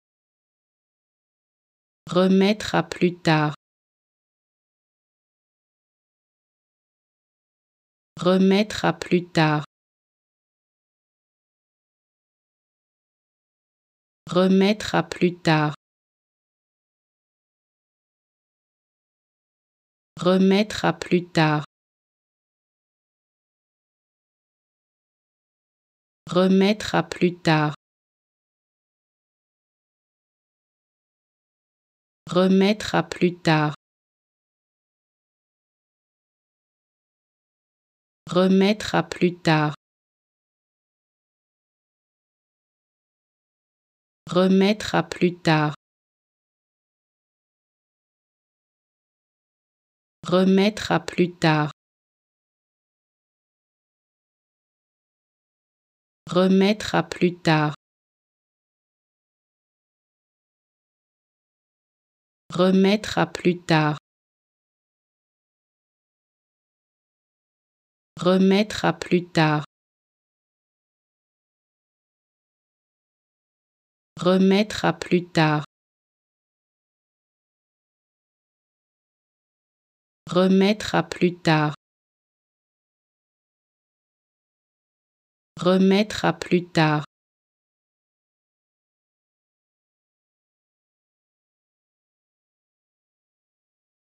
How to Pronounce ''Remettre à plus tard'' (Postpone) Correctly in French
Learn how to say and properly pronounce ''Remettre à plus tard'' in French with this free pronunciation tutorial.